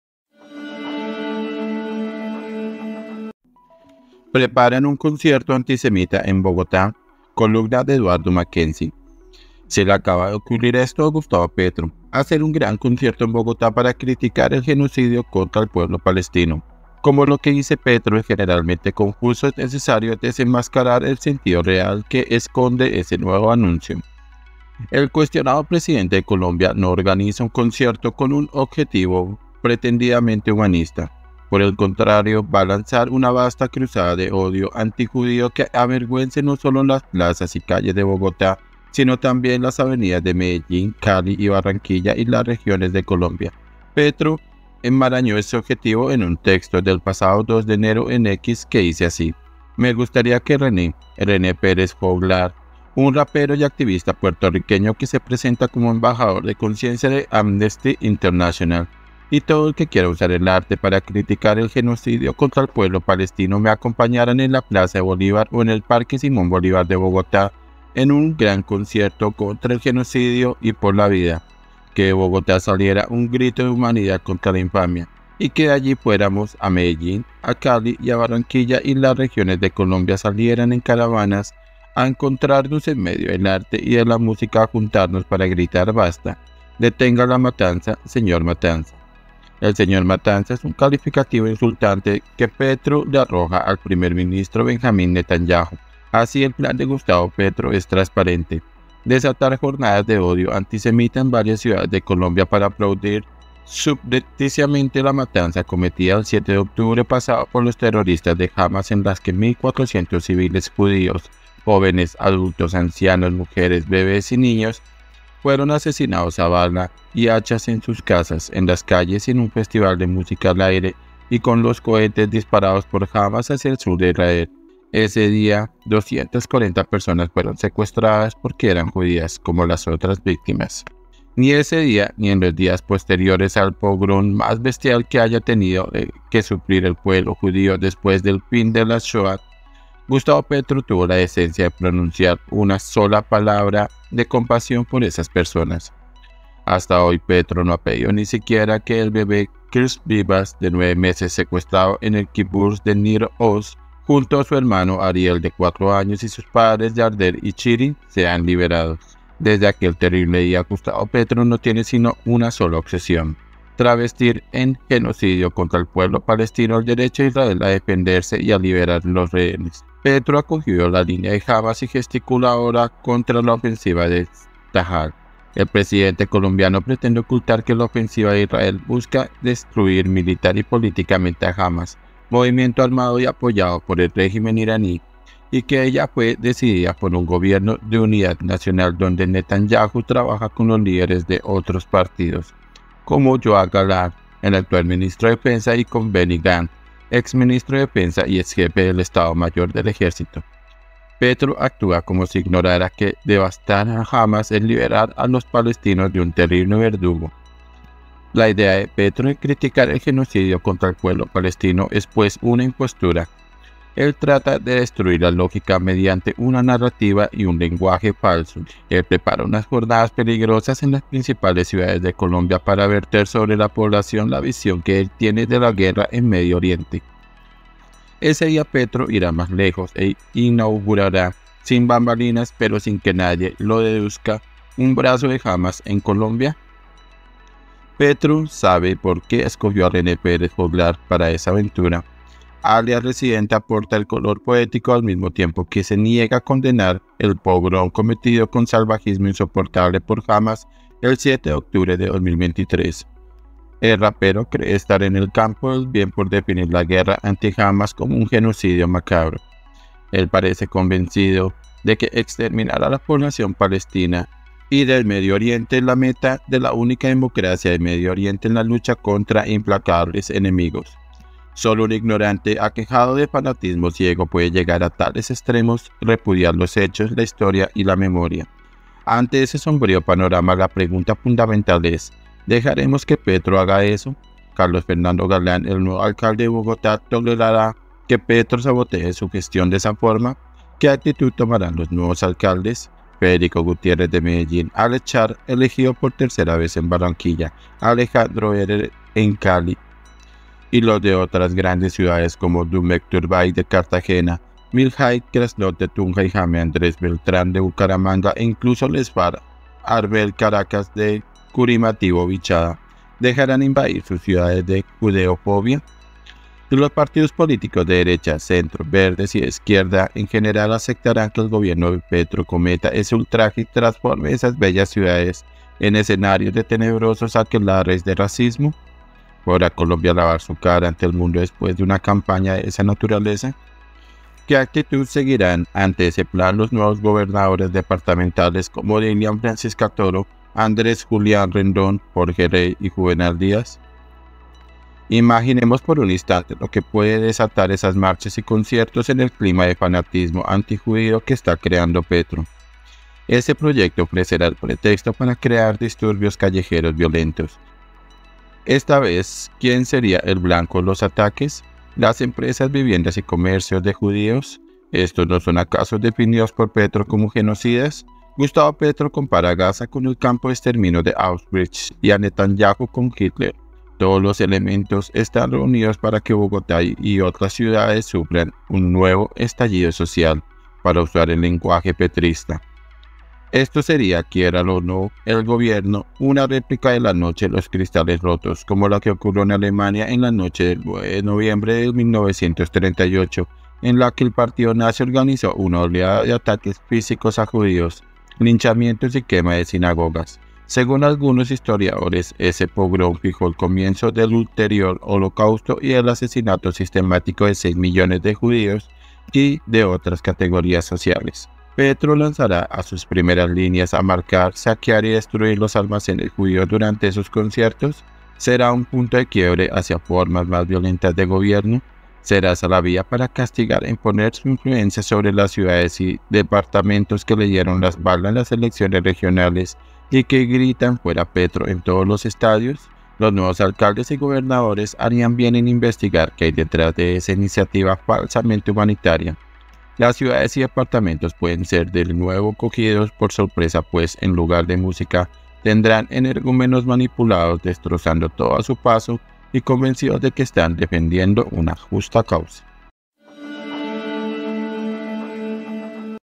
¿PREPARAN UN CONCIERTO ANTISEMITA EN BOGOTÁ? 🔵 Por Eduardo Mackenzie
El cuestionado presidente de Colombia no organiza un concierto con un objetivo pretendidamente humanista. Por el contrario, va a lanzar una vasta cruzada de odio antijudío que avergüence no solo las plazas y calles de Bogotá sino también las avenidas de Medellín, Cali y Barranquilla y “las regiones de Colombia”.